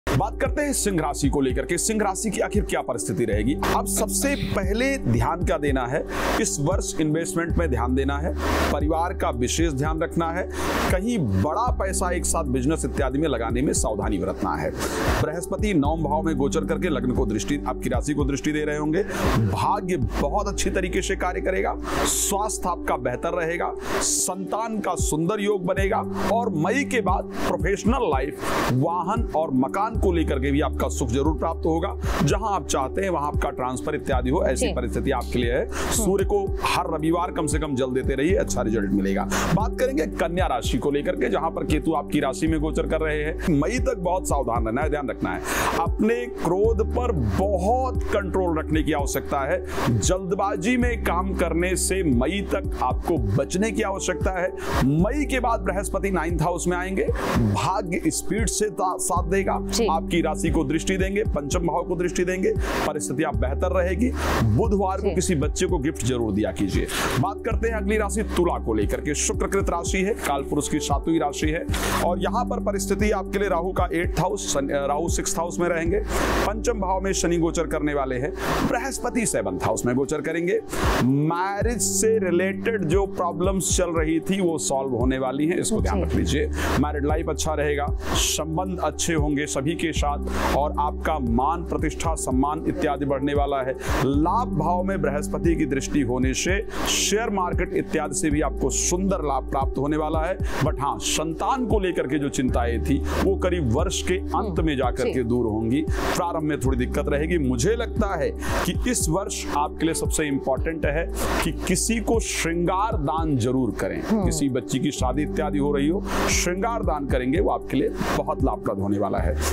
Year 2024:सिंह, कन्या और तुला राशि के लिए सबसे सटीक भविष्यवाणी, नये साल में होंगे मालामाल या बेहाल!
सिंह, कन्या और तुला  राशि के लिए सबसे सटीक भविष्यवाणी, नये साल में होंगे मालामाल या बेहाल!

नये साल 2024 का आगाज़ हो चुका है। हर कोई ये जानने के लिए बेहद उत्सुक है कि ये साल उनके लिए कैसा रहने वाला है।इस वीडियो में हम आपको बताएंगे सिंह, कन्या और तुला  राशि के बार में ।

The new year 2024 has started. Everyone is very curious to know how this year is going to be for them. In this video we will tell you about Leo, Virgo and Libra.